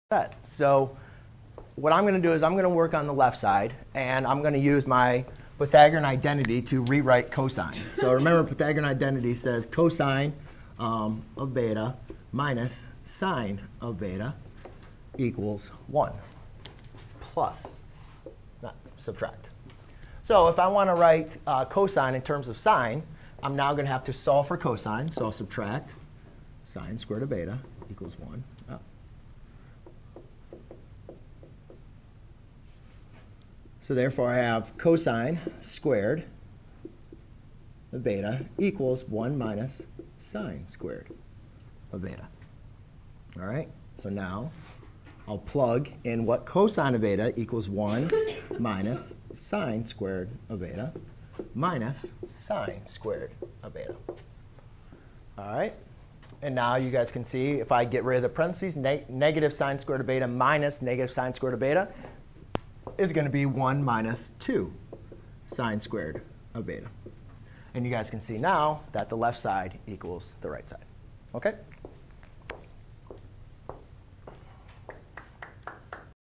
Using the Pythagorean identity to verify an identity
👉 Learn how to verify Pythagoras trigonometric identities. A Pythagoras trigonometric identity is a trigonometric identity of the form sin^2 (x) + cos^2 (x) or any of its derivations. To verify trigonometric expression means to verify that the term(s) on the left-hand side of the equality sign is equal to the term(s) on the right-hand side.

To verify Pythagoras trigonometric identities, we convert the given trigonometric identity to any of the forms of the Pythagoras trigonometric identity.